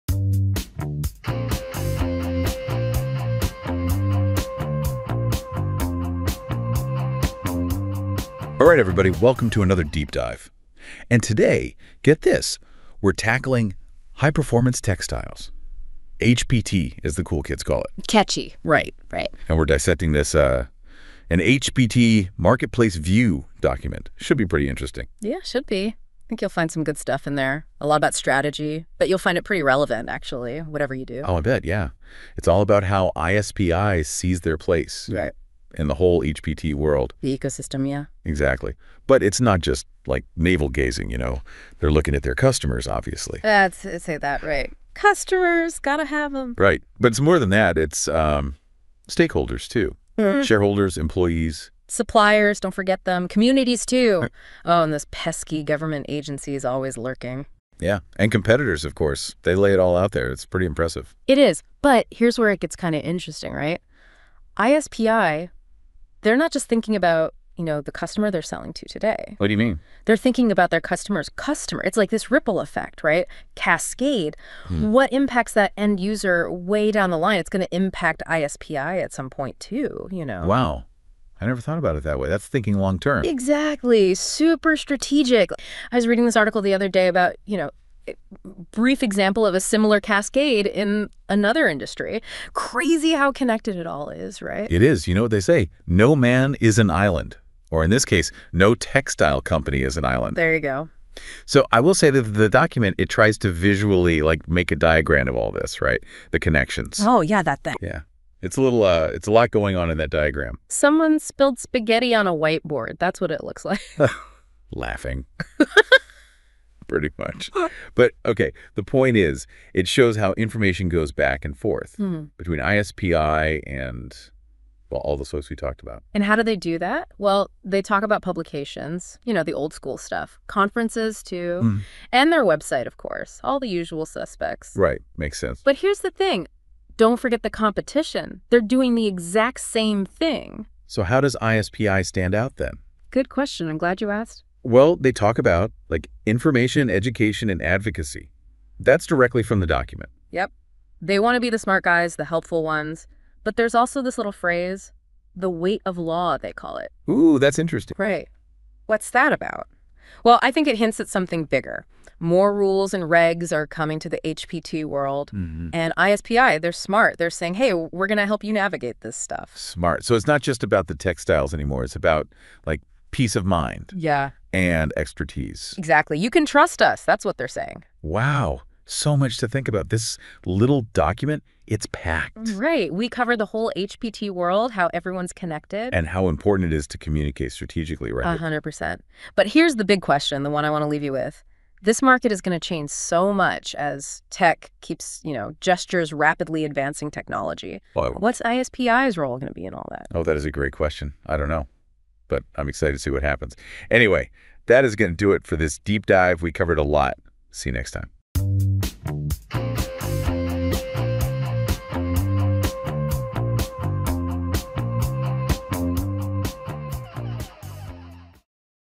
Notebook LM Podcast Review of An HPT Marketplace View 2000
The model presented in the source is intended to get us thinking about ISPI as "the primary source of information, education, and advocacy for HPT." The source notes that we don't exist in a vacuum and neither do our customers - it's a complex marketplace.

The model outlines the need for communication and education to ensure that the needs of all stakeholders are met. The source suggests that organizations within the HPT marketplace must consider the unique needs of different customers, choose the best channels for reaching those customers, and understand how their products and services can best address those needs.

The article also emphasizes the importance of working with government bodies and understanding the complex regulatory environment of the marketplace.

Resources

The Article
- An HPT Marketplace View